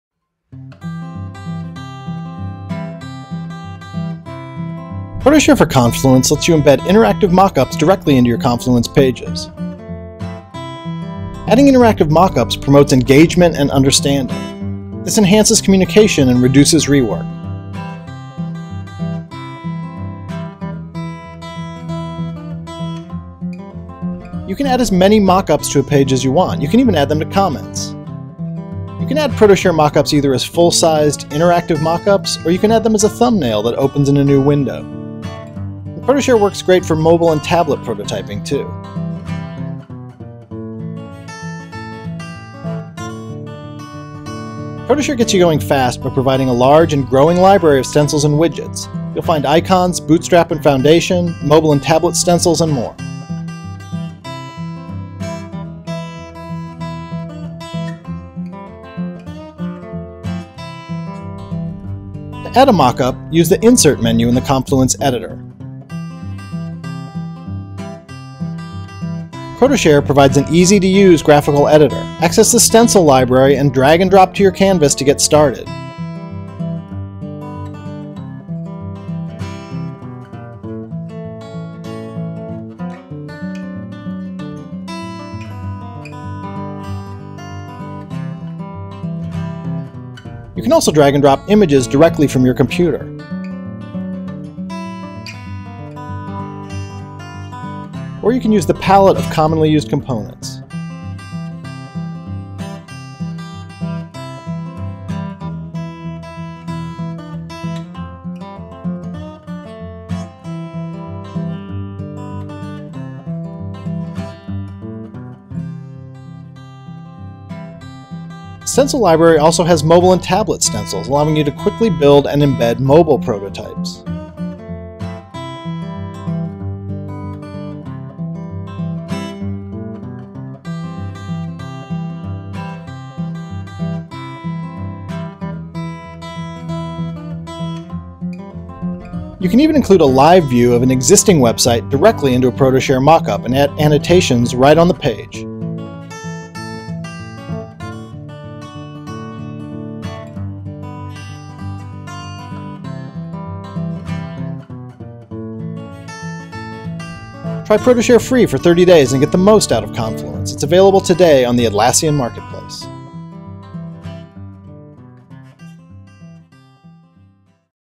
Wireframes for Confluence. ProtoShare Prototyping, Wireframing for Web and Mobile
Create mockups, wireframes and prototypes for websites, mobile and applications directly in Confluence using ProtoShare for confluence, a great website wireframing tool that lets you build low, medium and high-fidelity website prototypes. Use confluences collaboration, version control and other features to super-charge your development process.